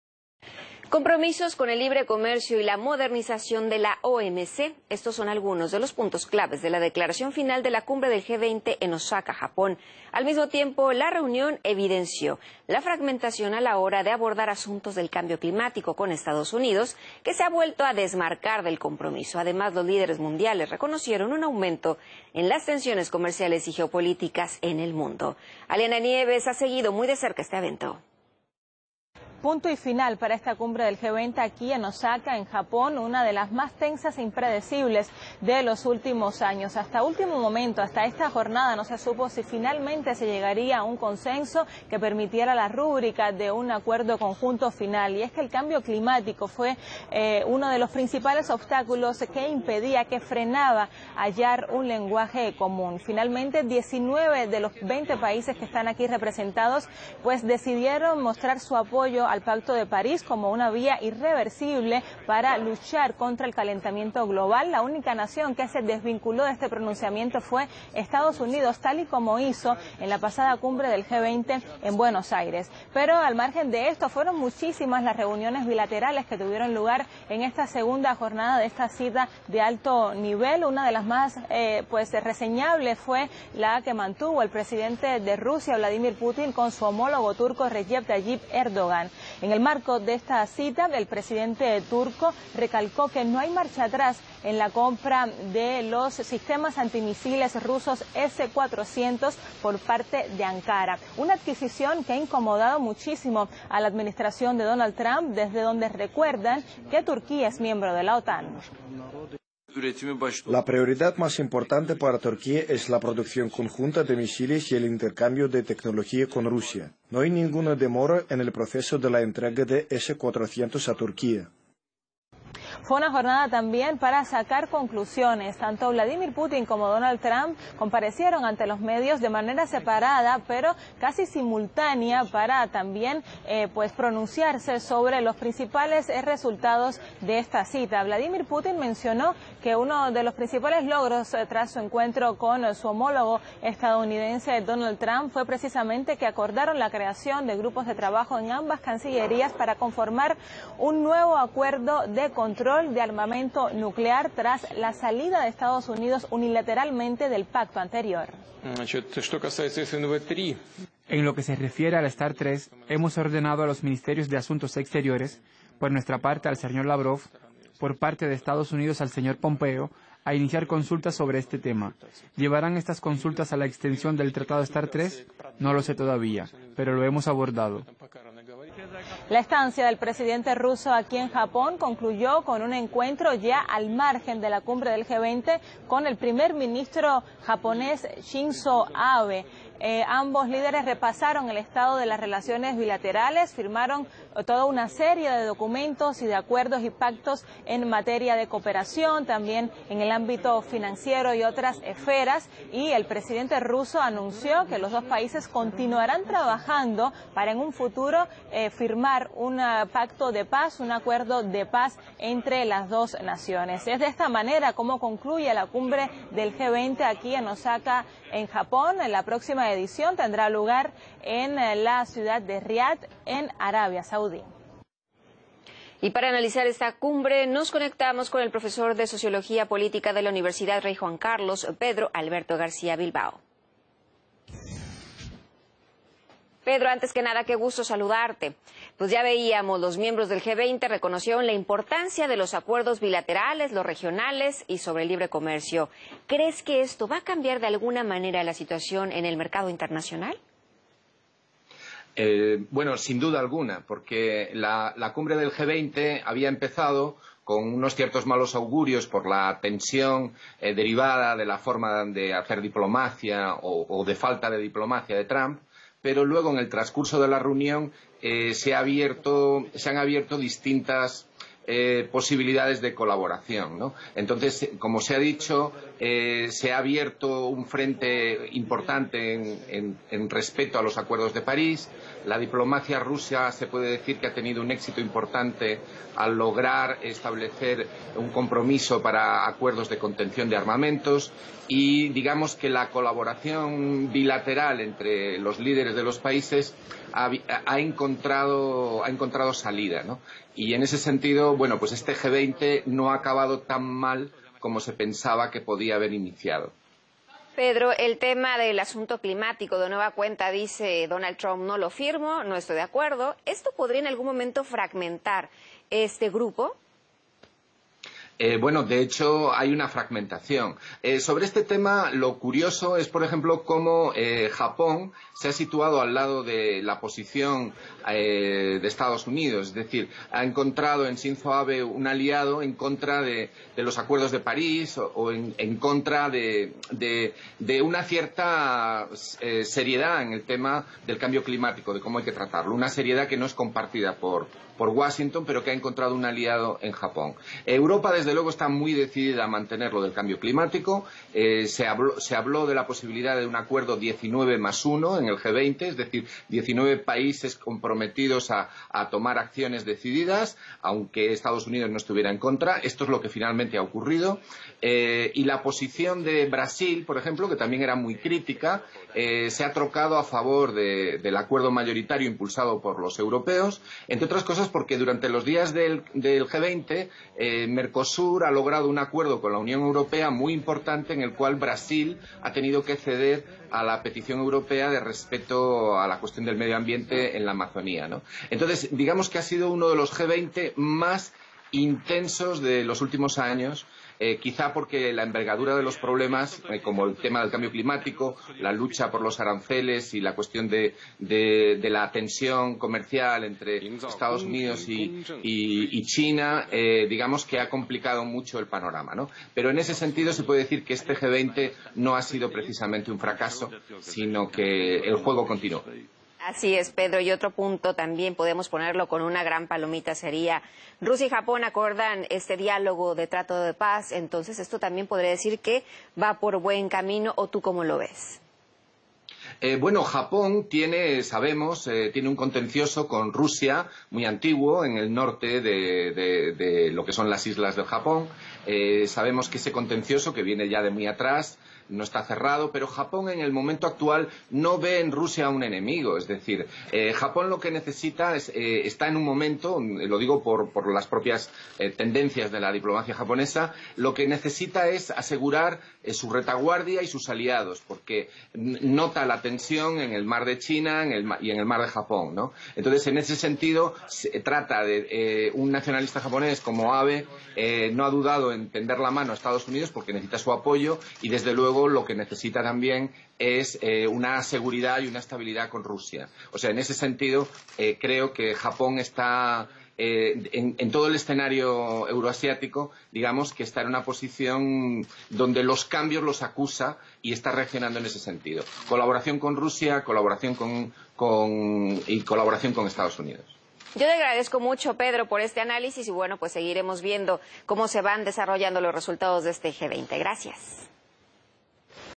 El G20 ratifica su compromiso con el pacto sobre el cambio climtico con la excepcin de EEUUyoutube c
Los compromisos con el libre comercio y la modernización de la OMC son algunos de los puntos clave de la declaración final del G20 en Osaka (Japón). La reunión puso de relieve la fragmentación sobre la cuestión del cambio climático. Los líderes mundiales reconocieron un aumento de las tensiones comerciales y geopolíticas. En opinión de Pedro Alberto García Bilbao, profesor de sociología política, el encuentro "no ha acabado tan mal como se pensaba".